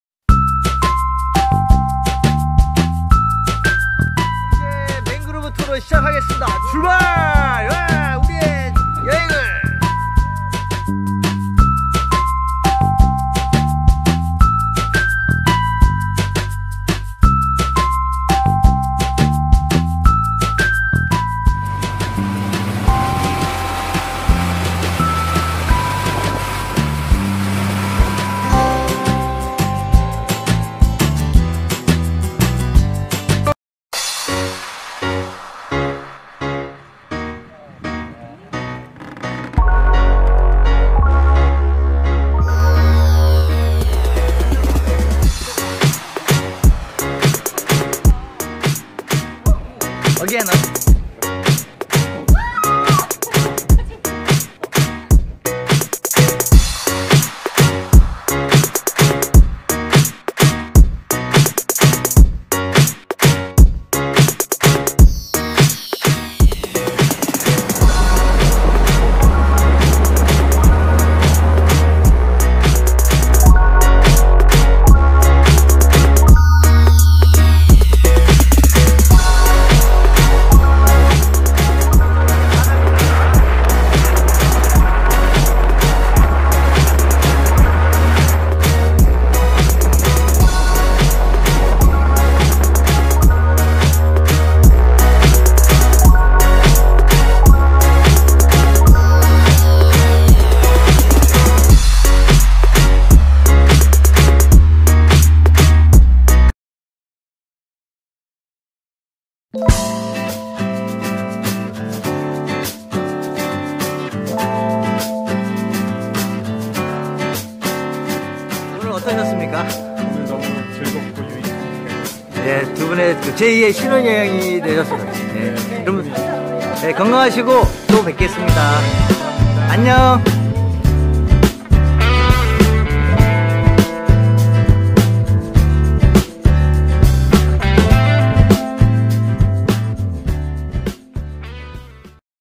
말레이시아 자유여행, 랑카위에서 두번째 신혼여행 허니문을 가다.
말레이시아 랑카위 자유여행 맹그로브 단독 투어 영상은 2분의 자연사랑 후원금으로 작성 되었습니다.  맹그로브 만큼 굳건한 사랑 하시기 바랍니다. ^^